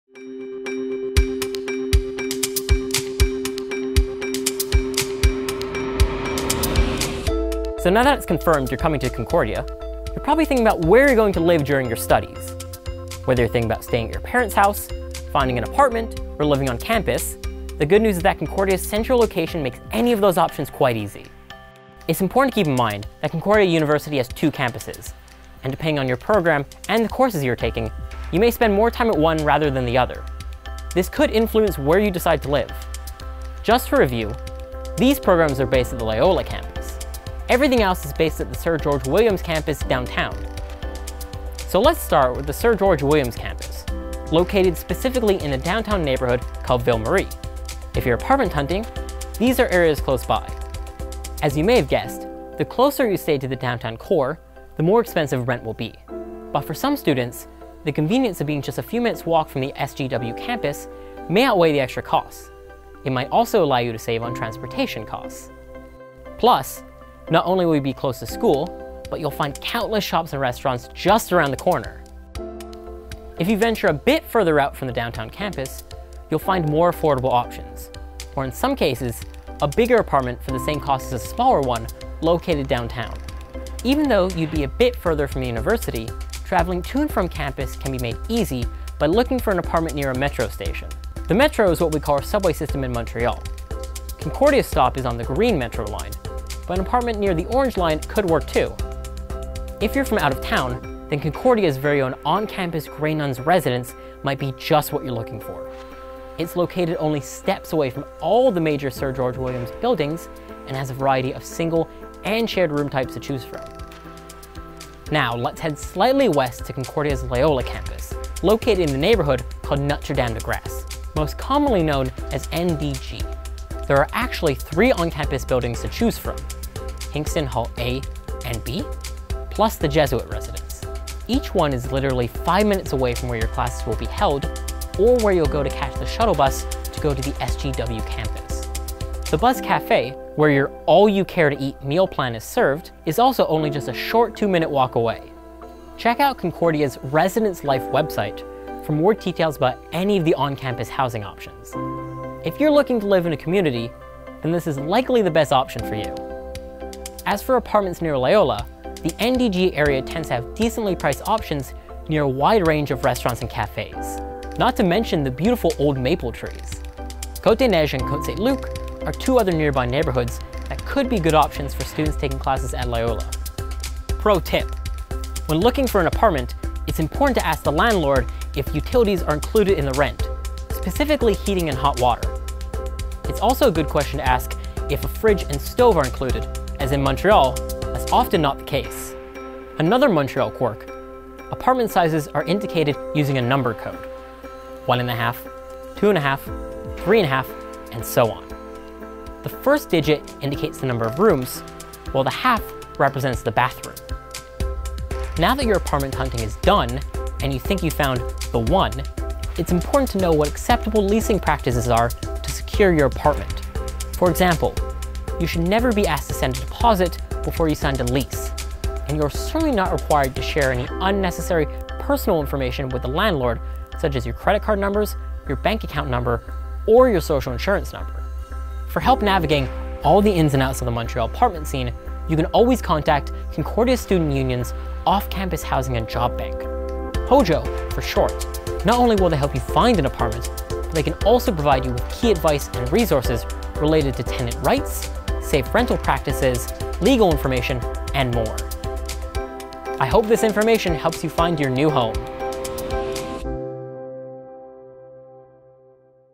On/Off Campus Housing
New to Montreal or moving into your own place for the first time? This video will give you a great introduction to housing options, whether it’s on campus in Concordia’s residences or off campus.